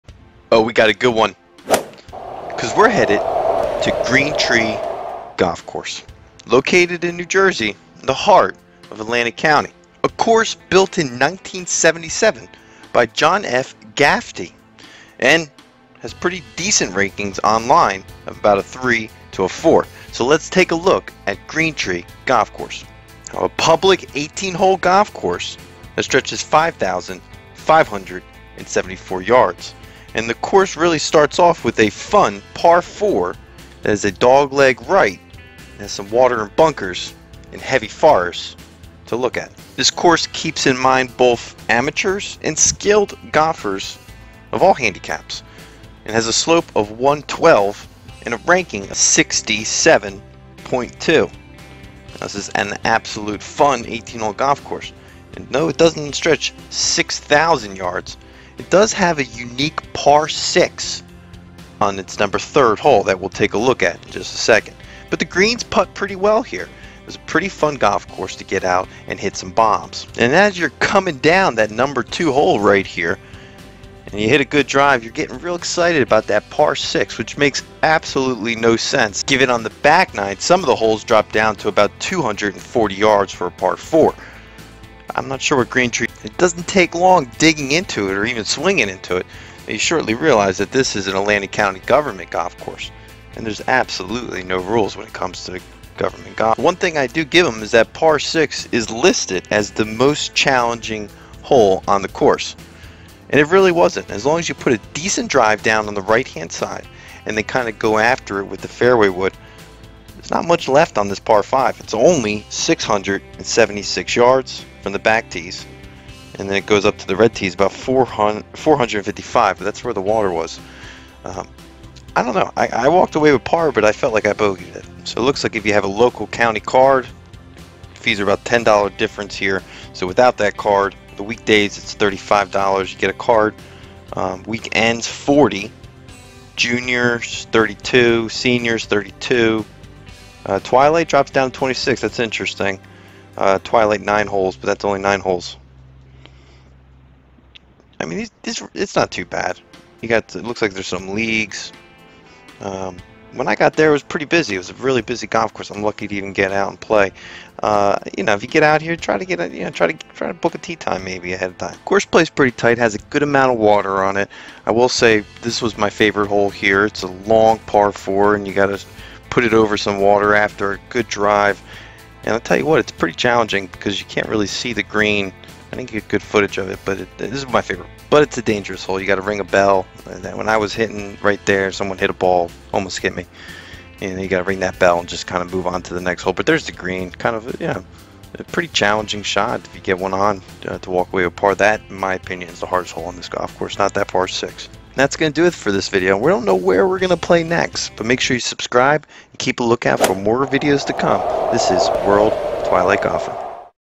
Green Tree Golf Course - John F. Gaffney #43
The video "Green Tree Golf Course - John F. Gaffney - Golf Vlog Review" on the Twilight Golfer channel is a candid review of the Green Tree Golf Course in New Jersey. The video features John F. Gaffney, a seasoned golfer, sharing his experiences and insights while playing a round of golf at Green Tree Golf Course.

The video starts with John arriving at the course and giving a brief introduction to the history and layout of the club. Throughout the video, John shares his honest opinions and feedback on the course, highlighting its strengths and weaknesses.

Viewers get to see John playing a round of golf, providing commentary on each hole, the challenges he faced, and the overall course conditions. He also shares tips and tricks for playing the course effectively, making the video informative for viewers looking to improve their game.

Throughout the video, viewers get a first-hand look at the course conditions, which John rates as a 4.3 out of 10. Despite the low rating, John offers constructive criticism and feedback, making the video valuable for the course owners and management.

The video also includes drone footage, providing a unique perspective of the entire layout of the course. After completing the round, John provides a detailed review of the Green Tree Golf Course, covering topics such as the course design, layout, course conditions, and amenities.

The video offers an open and engaging forum for golfers to share their own experiences and opinions on the course.

Overall, "Green Tree Golf Course - John F. Gaffney - Golf Vlog Review" provides a candid and informative review of the course, offering valuable feedback for the course owners and management, as well as an engaging viewing experience for golfers.